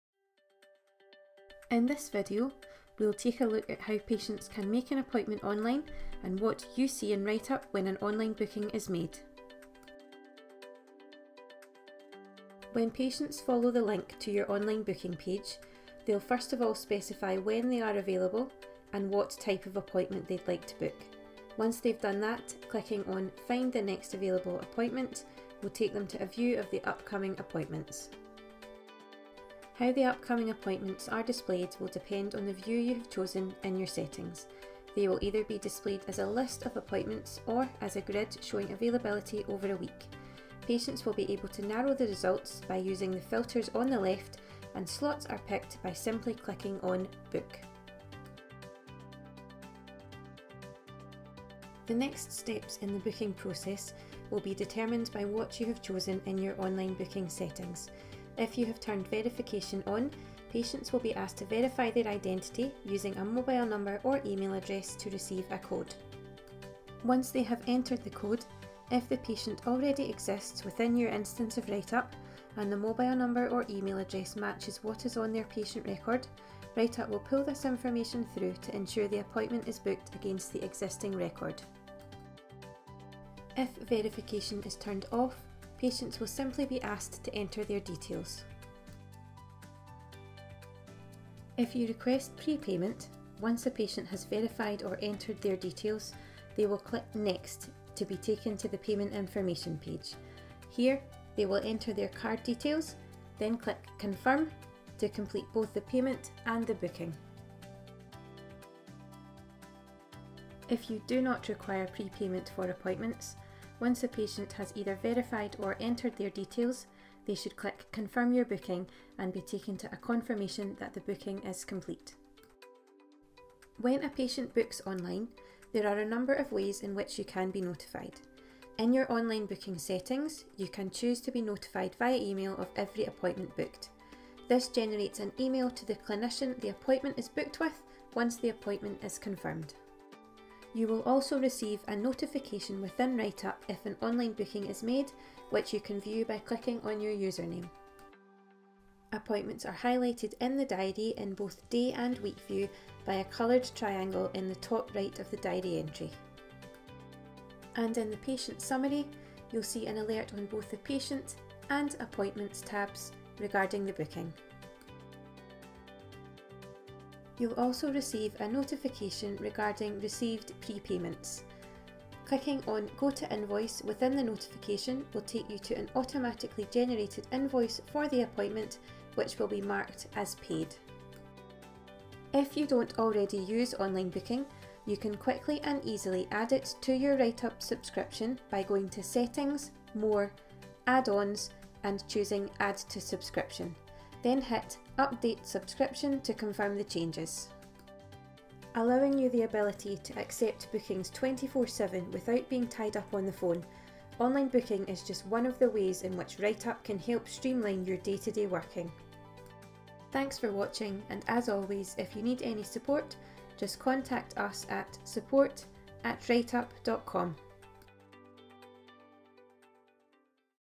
How Your Patients Use Online Booking in WriteUpp
A video guide showing how your patients make an appointment online, and what you see within WriteUpp when a booking is made.

Note - There may have been some changes to WriteUpp since this video was recorded so please do contact the team on live chat or from within your own WriteUpp account if you have any further questions!